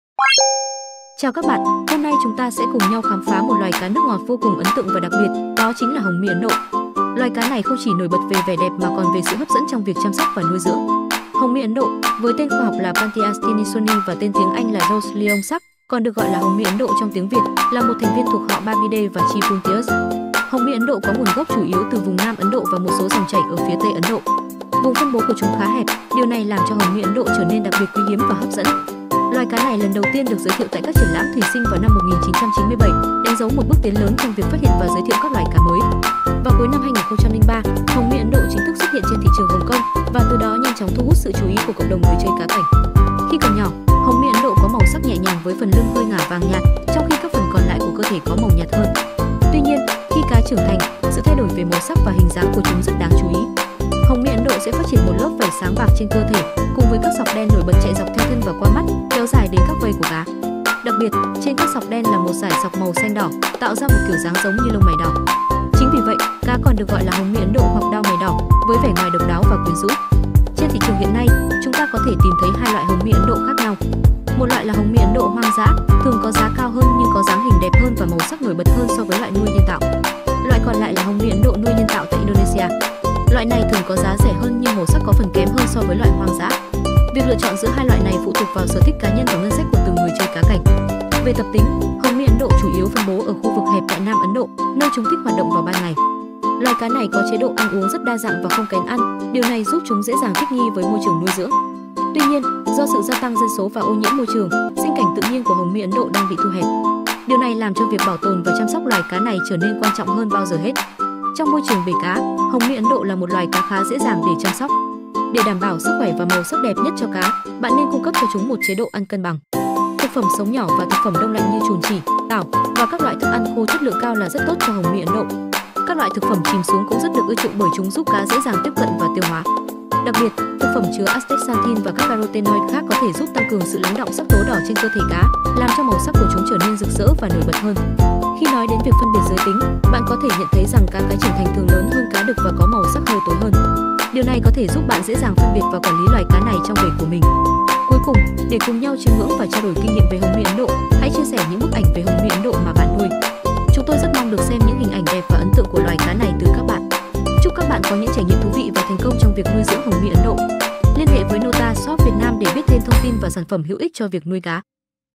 Tìm Hiểu Về Cá Hồng Mi Ấn: Loài Cá Đẹp Nổi Bật Trong Thủy Sinh!
Chào các bạn, hôm nay chúng ta sẽ cùng nhau khám phá một loài cá nước ngọt vô cùng ấn tượng và đặc biệt, đó chính là Hồng Mi Ấn Độ. Loài cá này không chỉ nổi bật về vẻ đẹp mà còn về sự hấp dẫn trong việc chăm sóc và nuôi dưỡng. Hồng Mi Ấn Độ, với tên khoa học là Pantias Tinisoni và tên tiếng Anh là Rose Lyon Sark, còn được gọi là Hồng Mi Ấn Độ trong tiếng Việt, là một thành viên thuộc họ Barbidae và chi Puntius. Hồng Mi Ấn Độ có nguồn gốc chủ yếu từ vùng nam Ấn Độ và một số dòng chảy ở phía tây Ấn Độ. Vùng phân bố của chúng khá hẹp, điều này làm cho Hồng Mi Ấn Độ trở nên đặc biệt quý hiếm và hấp dẫn. Loài cá này lần đầu tiên được giới thiệu tại các triển lãm thủy sinh vào năm 1997, đánh dấu một bước tiến lớn trong việc phát hiện và giới thiệu các loài cá mới. Vào cuối năm 2003, Hồng Mi Ấn Độ chính thức xuất hiện trên thị trường Hong Kong, và từ đó nhanh chóng thu hút sự chú ý của cộng đồng người chơi cá cảnh. Khi còn nhỏ, Hồng Mi Ấn Độ có màu sắc nhẹ nhàng với phần lưng hơi ngả vàng nhạt, trong khi các phần còn lại của cơ thể có màu nhạt hơn. Tuy nhiên, khi cá trưởng thành, sự thay đổi về màu sắc và hình dáng của chúng rất đáng chú ý. Hồng Mi Ấn Độ sẽ phát triển một lớp vảy sáng bạc trên cơ thể, cùng với các sọc đen nổi bật chạy dọc theo thân và qua mắt, kéo dài đến các vây của cá. Đặc biệt, trên các sọc đen là một dải sọc màu xanh đỏ, tạo ra một kiểu dáng giống như lông mày đỏ. Chính vì vậy, cá còn được gọi là Hồng Mi Ấn Độ hoặc Đao Mày Đỏ, với vẻ ngoài độc đáo và quyến rũ. Trên thị trường hiện nay, chúng ta có thể tìm thấy hai loại Hồng Mi Ấn Độ khác nhau. Một loại là Hồng Mi Ấn Độ hoang dã, thường có giá cao hơn nhưng có dáng hình đẹp hơn và màu sắc nổi bật hơn so với loại nuôi nhân tạo. Loại còn lại là Hồng Mi Ấn Độ nuôi nhân tạo tại Indonesia. Loại này thường có giá rẻ hơn nhưng màu sắc có phần kém hơn so với loại hoang dã. Việc lựa chọn giữa hai loại này phụ thuộc vào sở thích cá nhân và ngân sách của từng người chơi cá cảnh. Về tập tính, Hồng Mi Ấn Độ chủ yếu phân bố ở khu vực hẹp tại nam Ấn Độ, nơi chúng thích hoạt động vào ban ngày. Loài cá này có chế độ ăn uống rất đa dạng và không kén ăn, điều này giúp chúng dễ dàng thích nghi với môi trường nuôi dưỡng. Tuy nhiên, do sự gia tăng dân số và ô nhiễm môi trường, sinh cảnh tự nhiên của Hồng Mi Ấn Độ đang bị thu hẹp. Điều này làm cho việc bảo tồn và chăm sóc loài cá này trở nên quan trọng hơn bao giờ hết. Trong môi trường bể cá, Hồng Mi Ấn Độ là một loài cá khá dễ dàng để chăm sóc. Để đảm bảo sức khỏe và màu sắc đẹp nhất cho cá, bạn nên cung cấp cho chúng một chế độ ăn cân bằng. Thực phẩm sống nhỏ và thực phẩm đông lạnh như trùn chỉ, tảo, và các loại thức ăn khô chất lượng cao là rất tốt cho Hồng Mi Ấn Độ. Các loại thực phẩm chìm xuống cũng rất được ưa chuộng bởi chúng giúp cá dễ dàng tiếp cận và tiêu hóa. Đặc biệt, thực phẩm chứa astaxanthin và các carotenoid khác có thể giúp tăng cường sự lắng đọng sắc tố đỏ trên cơ thể cá, làm cho màu sắc của chúng trở nên rực rỡ và nổi bật hơn. Khi nói đến việc phân biệt giới tính, bạn có thể nhận thấy rằng cá cái trưởng thành thường lớn hơn cá đực và có màu sắc hơi tối hơn. Điều này có thể giúp bạn dễ dàng phân biệt và quản lý loài cá này trong bể của mình. Cuối cùng, để cùng nhau chiêm ngưỡng và trao đổi kinh nghiệm về Hồng Mi Ấn Độ, hãy chia sẻ những bức ảnh về Hồng Mi Ấn Độ mà bạn nuôi. Chúng tôi rất mong được xem những hình ảnh đẹp và ấn tượng của loài cá này từ các bạn. Chúc các bạn có những trải nghiệm thú vị và thành công trong việc nuôi dưỡng Hồng Mi Ấn Độ!

🔔 Đừng quên bật thông báo để không bỏ lỡ những video hấp dẫn từ chúng tôi! Hãy cùng nhau xây dựng một cộng đồng yêu thủy sinh đam mê và sáng tạo. 🌊🌿🐟